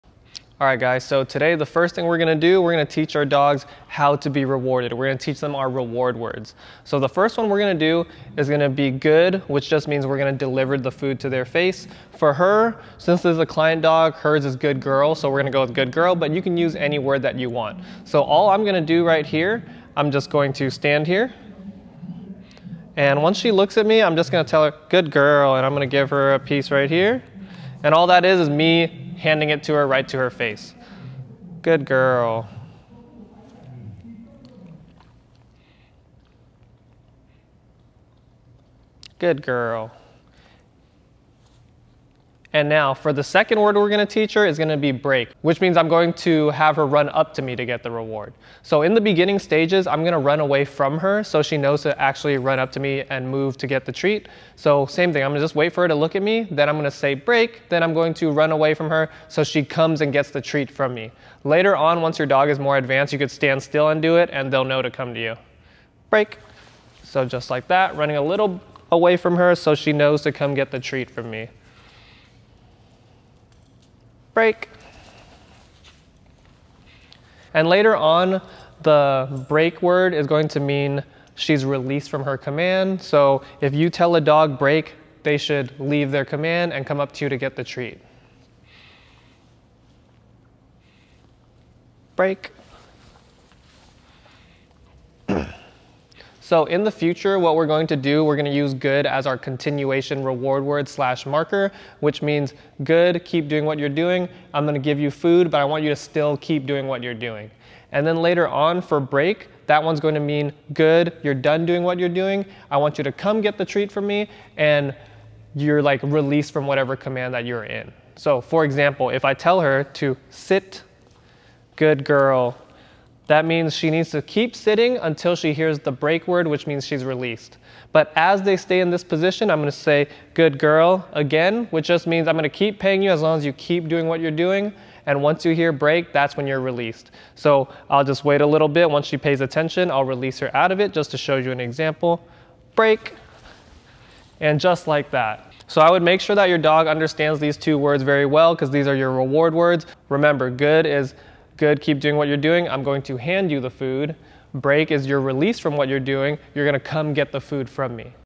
1. How to Reward Your Dog (Rewards & Marker Words)
Make sure to have different words for your continuation reward word and your release word! This will be the prerequisite to teaching your dog to continue commands until released such as stays, walking, recall, etc! This is the most foundational part of training